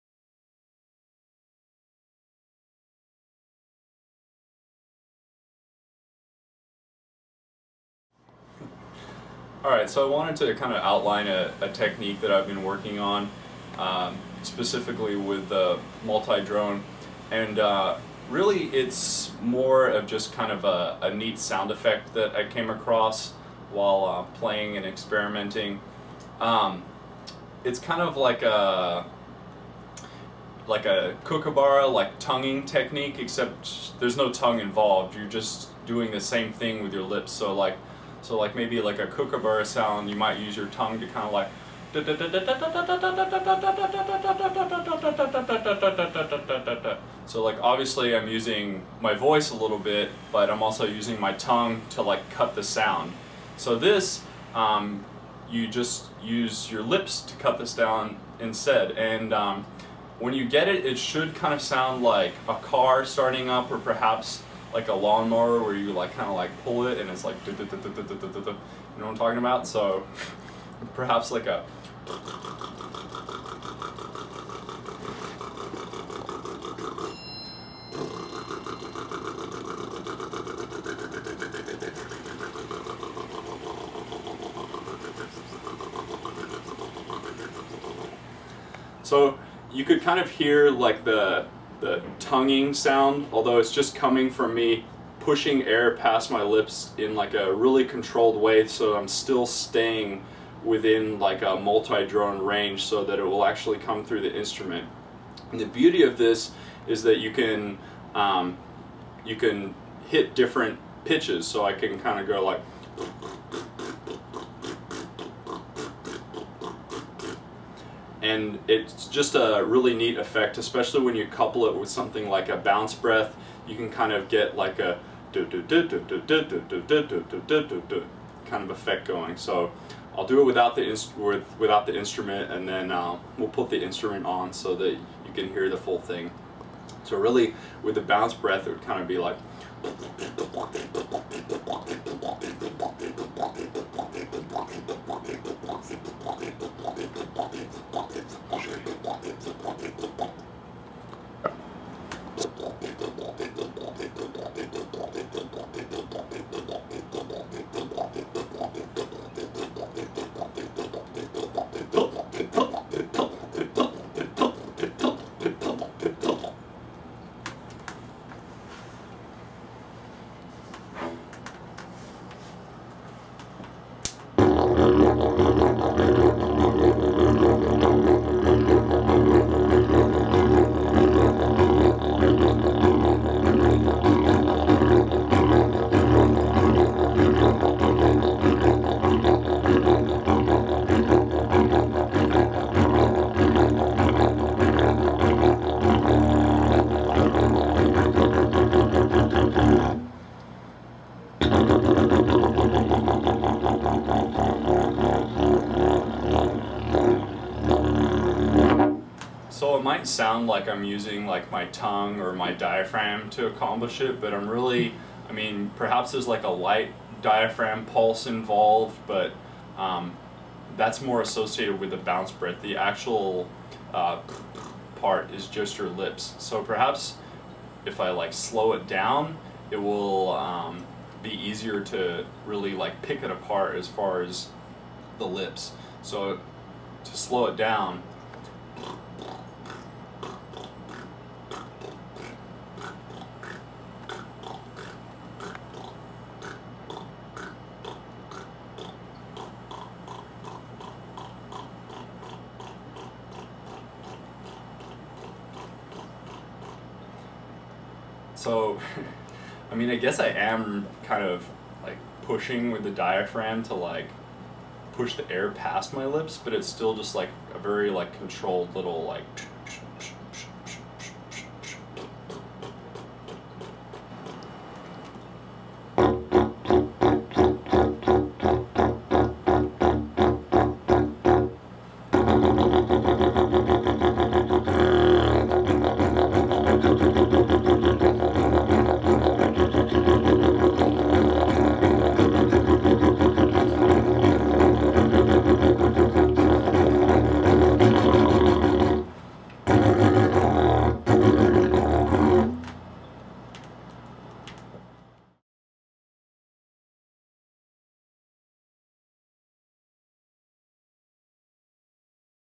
Ben Malcolm Teaches the Multi Drone Lip Pulse
Ben Teaches a technique he is developing around pulsing your lips to create a diaphragm push sounding pulse on the drop octave note of a multi drone didgeridoo.

The didgeridoo he is playing is a bamboo Multi Drone painted by aboriginal artist Lewis Burns

Filmed at WET studios, Los Angeles CA